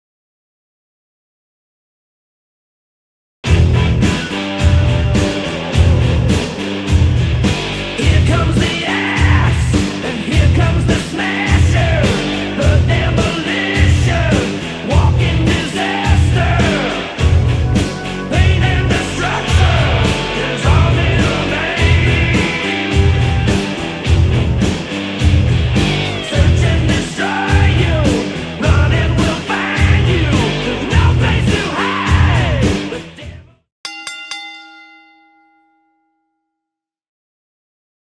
WWF Hasbro Custom Demolition Helmet
Here you can see my 3D printed Demolition helmets. They fit all three figures and are available with flexible and hard plastic.